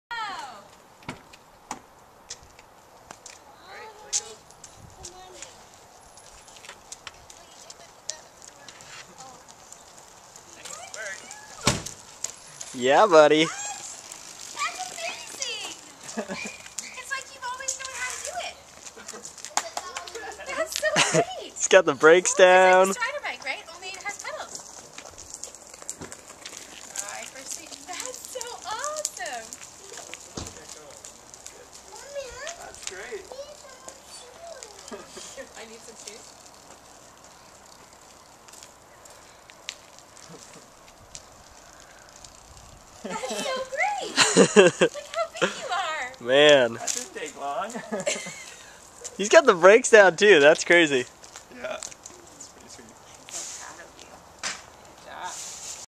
Silas riding without training wheels
I had taken Silas' training wheels off a couple times for a few short sessions in the past, but this time he really got it! The training wheels will be off for good now. I love the perma-smile on his face in this video.